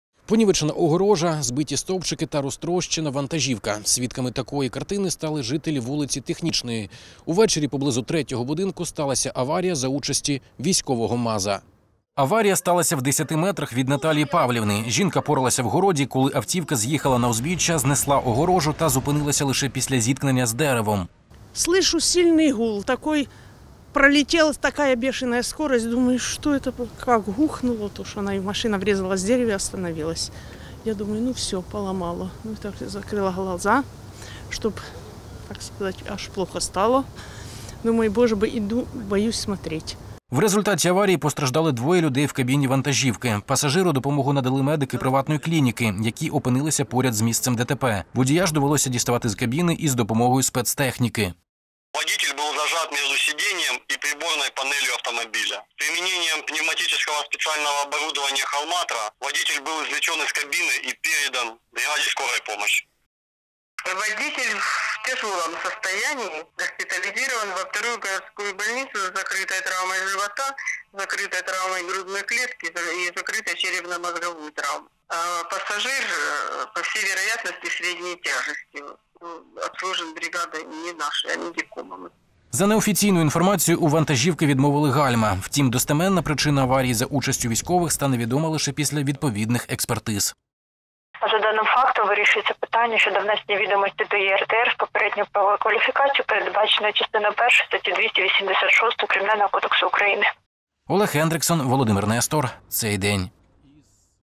ДТП з вантажівкою військових
Сьогодні в Саксаганському районі сталася дорожньо-транспортна пригода. Постраждало двоє людей. Подробиці - в сюжеті.